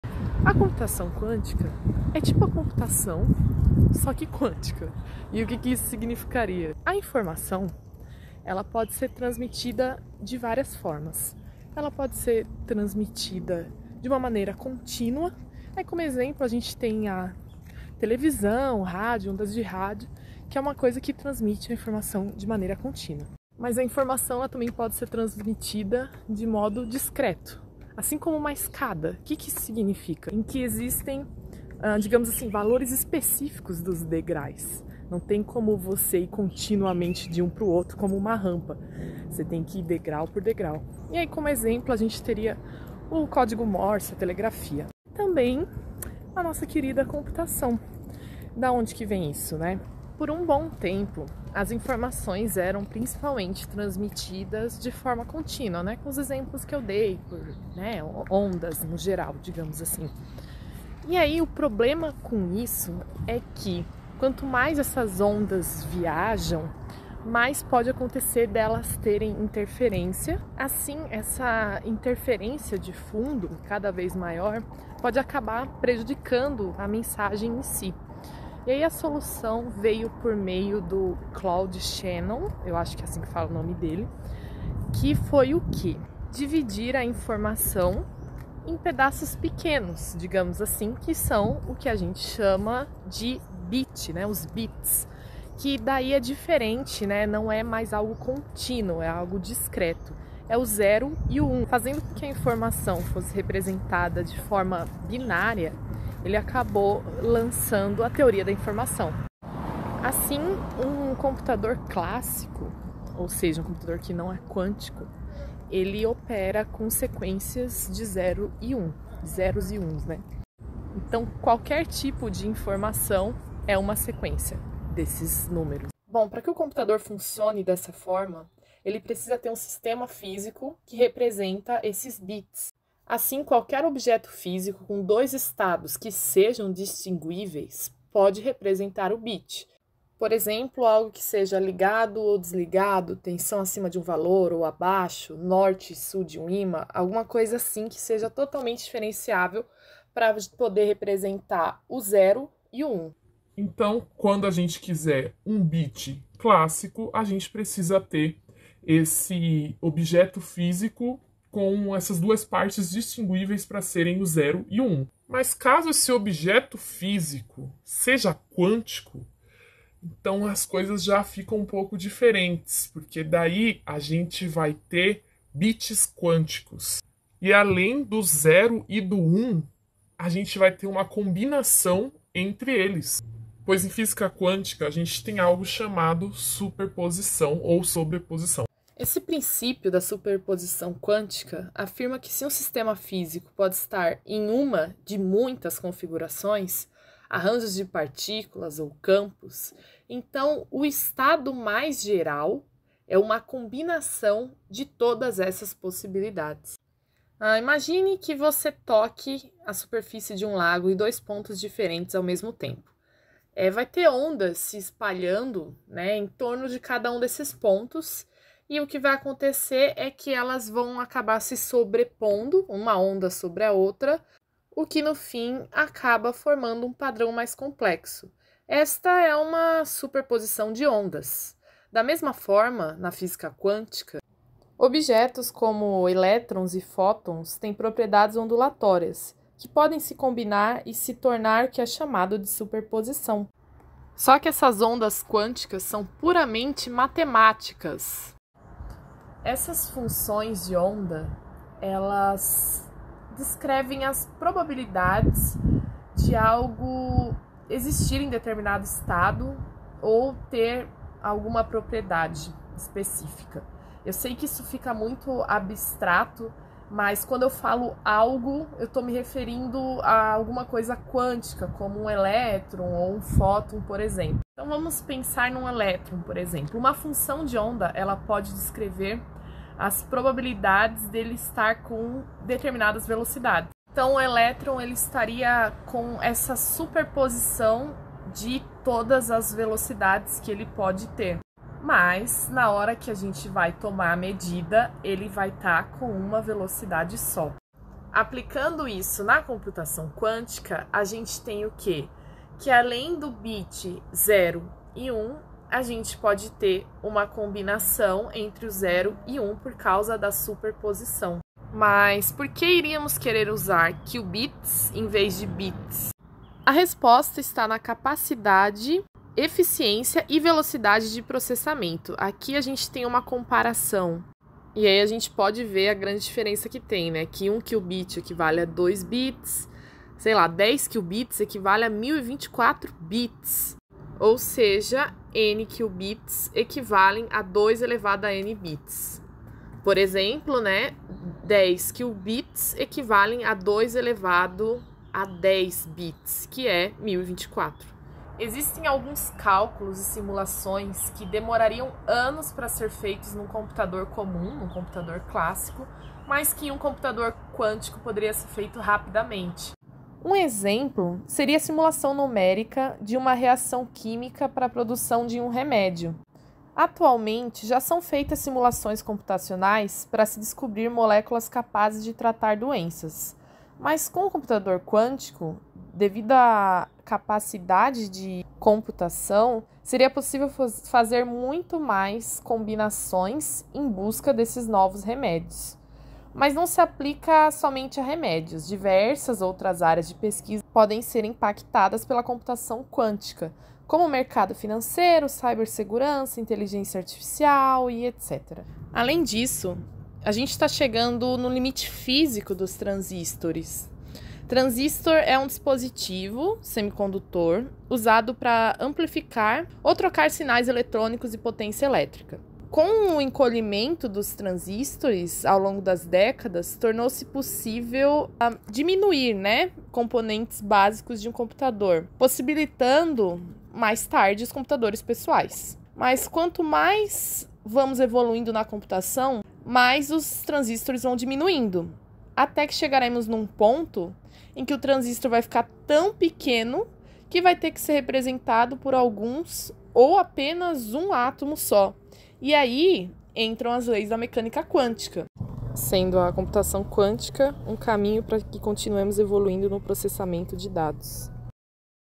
Noções gerais de computação quantica
AAAAH FALEI DEGRAIS EM VEZ DE DEGRAUS. Mas fora isso, o vídeo está bem satisfatório. O roteiro foi bem trabalhoso para escrever, demorei bastante hehe, contei com a ajuda do professor Roberto Sarthour da CBPF que entende do assunto para ver se eu não estava escrevendo besteira no roteiro. Se eventualmente houver algum outro erro é devido a informalidade da minha explicação.